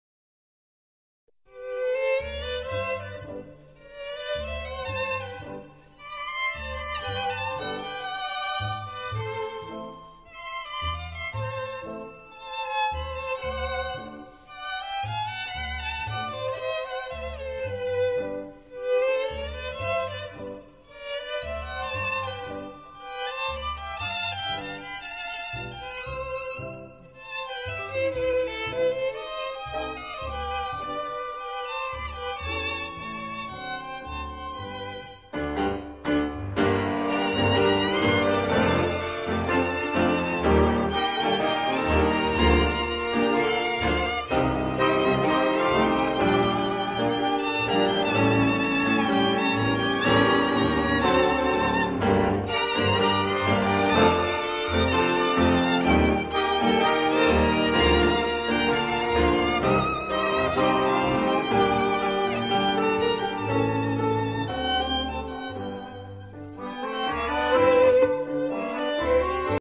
TDP 5 Tablet Press Machine how to adjust the thickness of tablet
How to adjust the thickness of tablet output from TDP 5 Tablet Press Machine